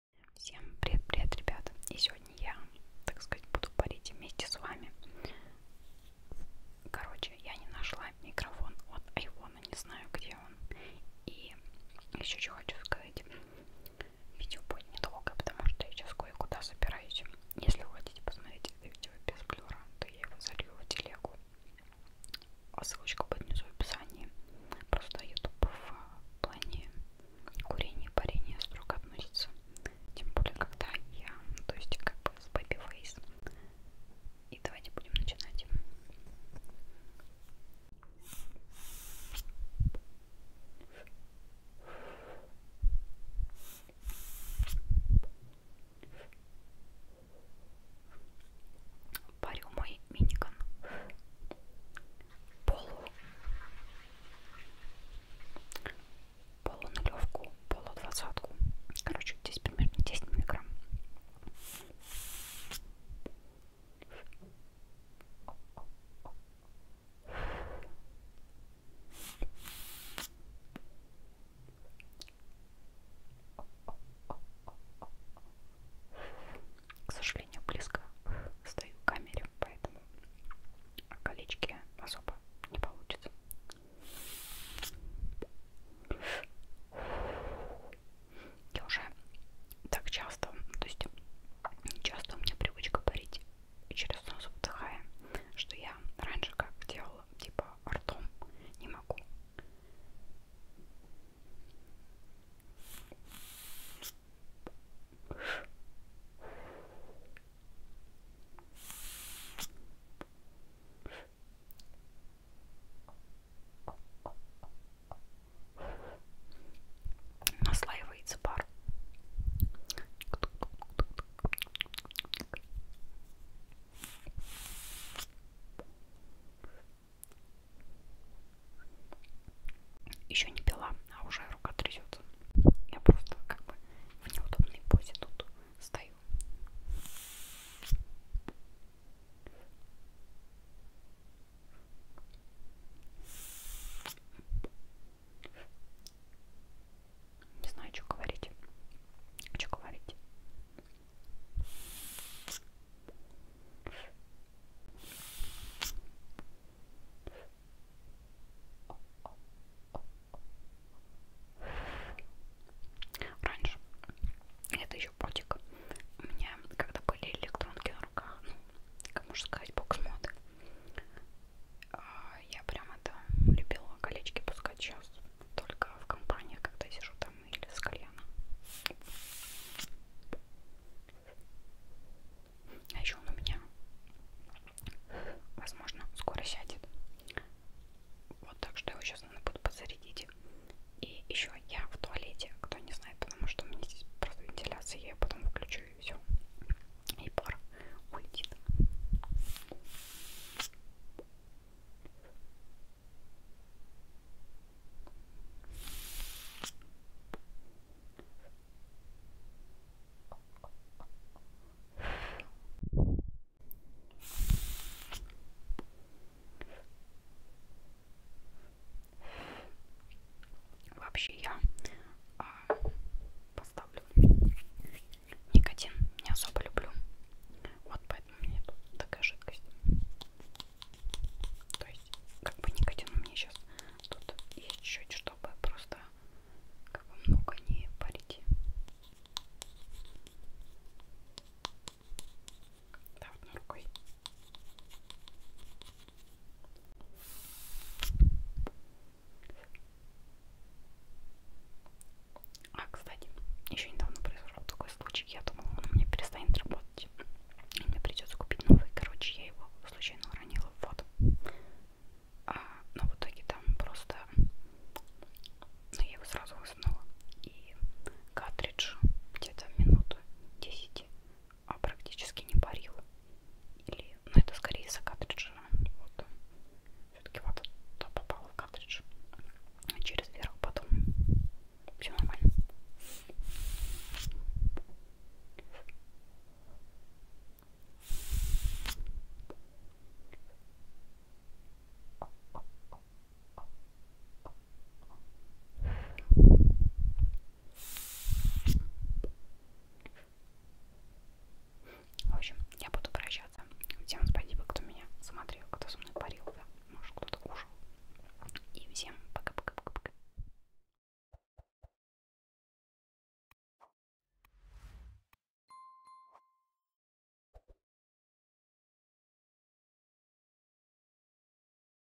АСМР ПАРЮ С ТОБОЙ В ПЕРЕРЫВЕ МЕЖДУ ПАРАМИ✌🏻😁💨
всем привет)
зовут меня Вика
мне 18 лет
если комментарии закрыты-это ютуб, а не я